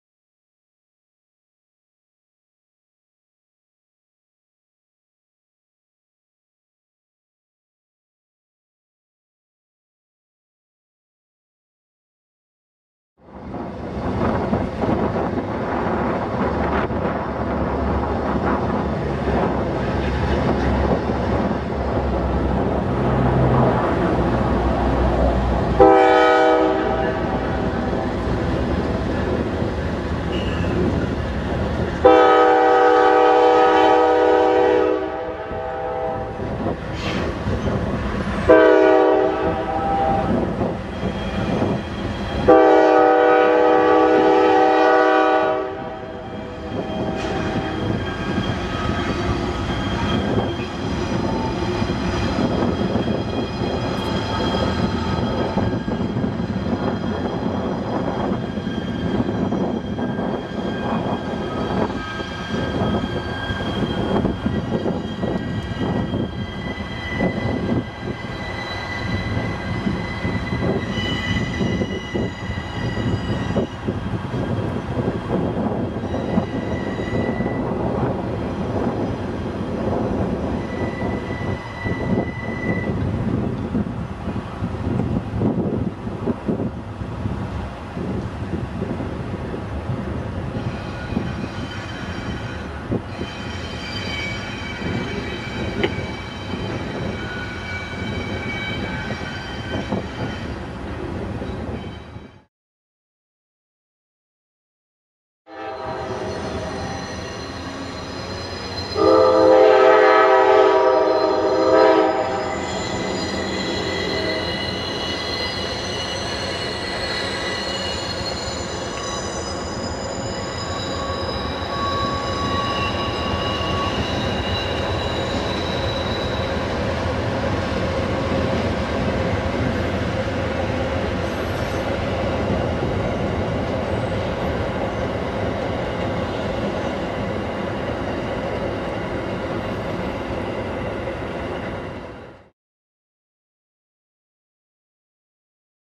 NS & Amtrak on the D&H
NS & Amtrak at the bridge crossing Maxon RD in Schenectady, NY. We are on the Scotia side of the Mohawk River.

Train 1:
NS 23K Inter Modal Freight

Train 2:
Amtrak Adirondack # 69